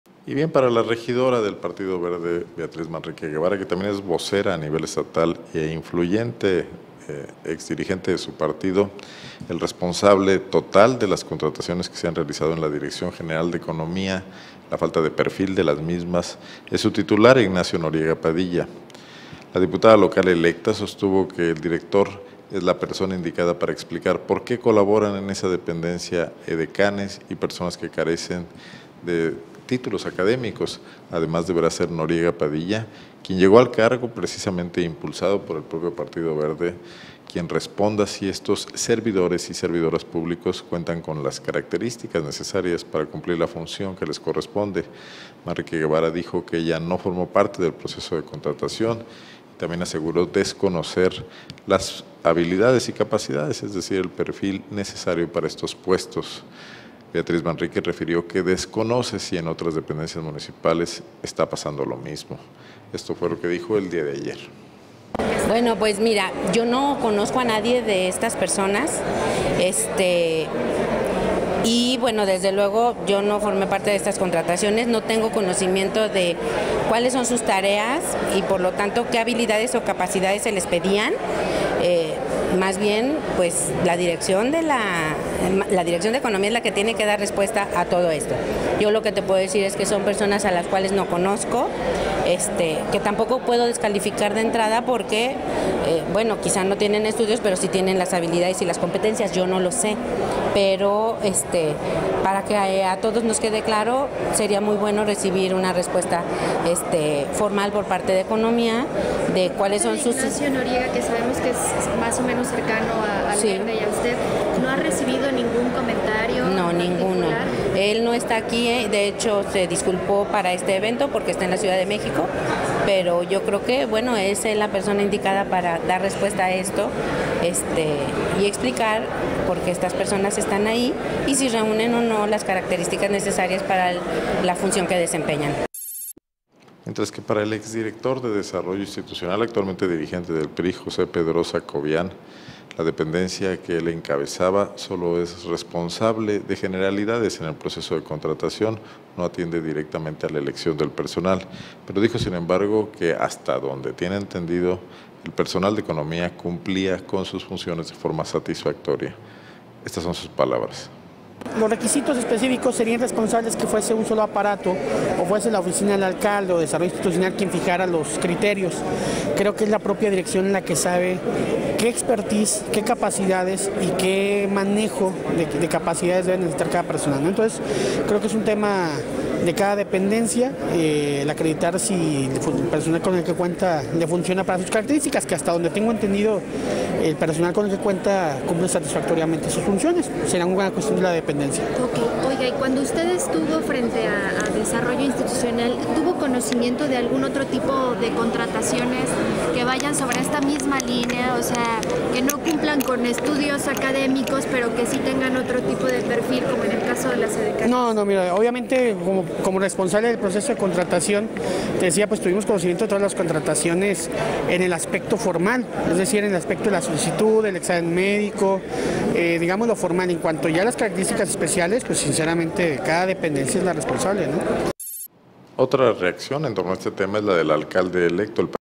Ignacio Noriega único responsable de contratación de edecanes: Beatriz Manrique
Para la regidora y lideresa del Partido Verde Ecologista de México,
Beatriz Manrique Guevara, el responsable de las contrataciones que se
han realizado en la Dirección General de Economía es el titular de la
misma, Ignacio Noriega Padilla.